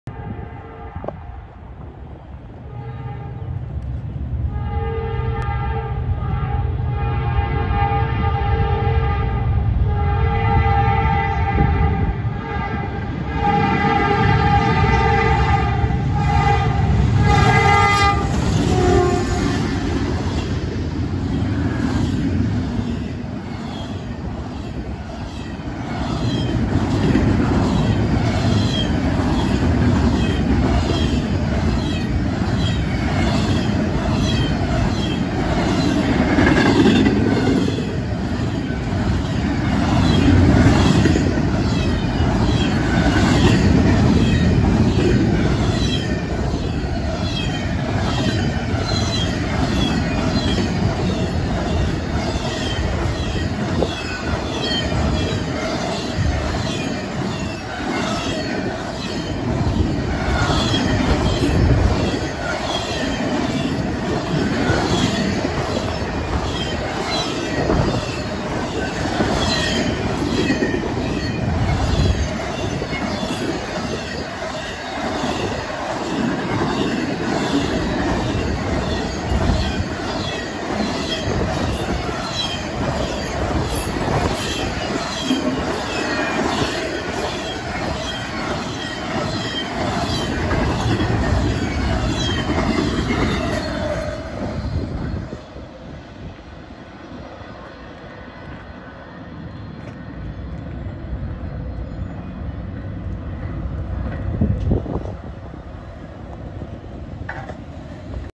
Springtime railfanning!! Norfolk Southern intermodal passing eastbound thru Chesterton, Indiana.
I was sitting at the Calumet Rd crossing in downtown Chesterton, Indiana when this Norfolk Southern intermodal came racing thru town east on the Chicago Line heading for Cleveland, Ohio.